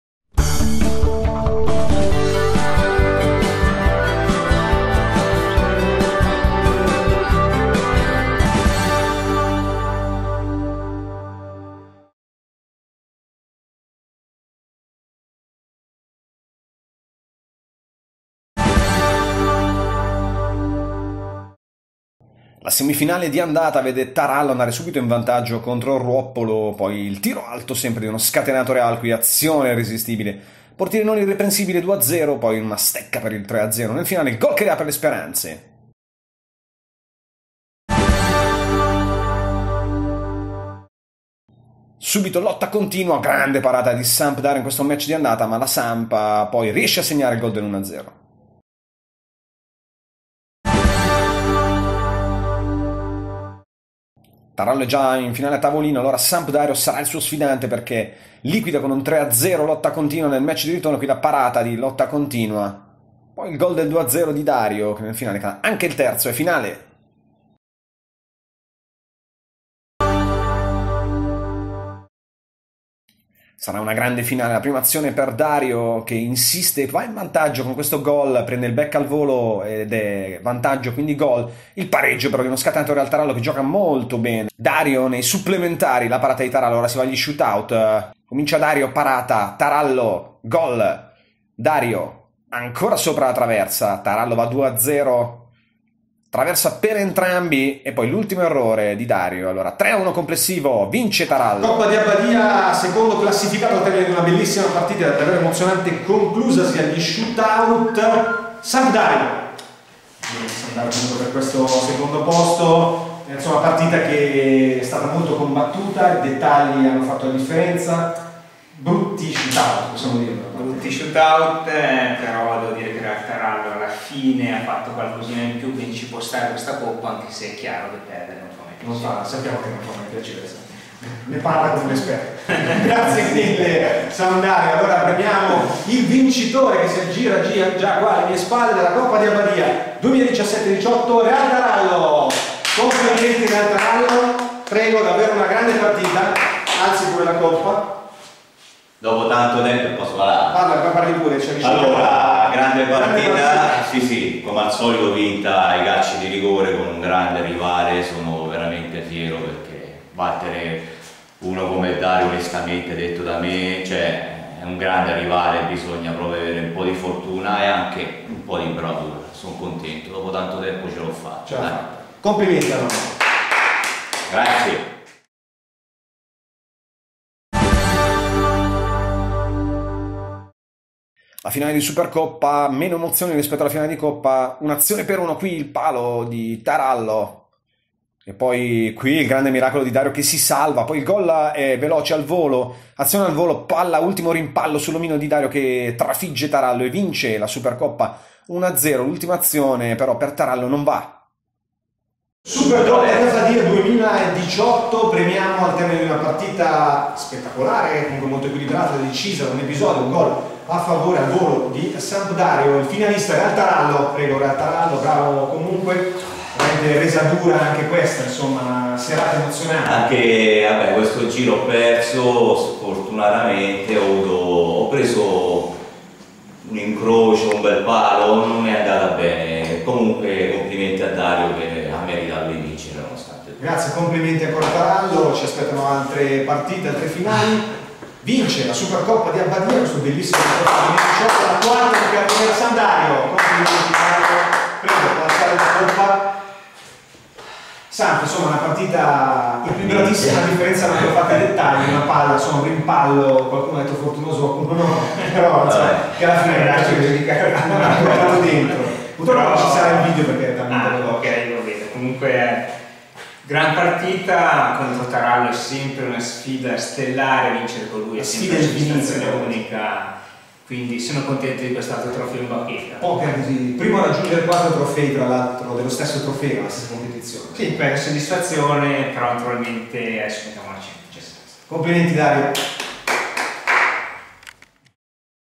2017/18 Coppa di Abbadia Semifinali, Finale | Supercoppa
Oltre il 30° minuto Coppa di Abbadia fasi finali con premiazioni + Supercoppa di Abbadia con premiazioni